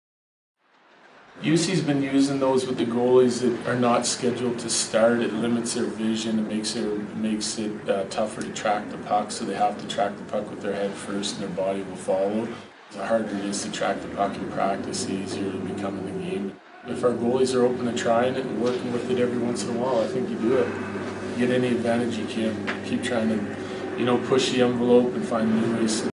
Coach Jussi Parkkila of the Colorado Avalanche Trains Goalies with Swivel Vision
Jussi Parkkila, NHL coach for the Colorado Avalanche, trains his goalies with Swivel Vision sports training aid. Take your game to the next level like Jussi and learn more Watch as he works to improve his athlete's focus, hand-eye coordination, and reaction time with the Swivel Vision for athletes.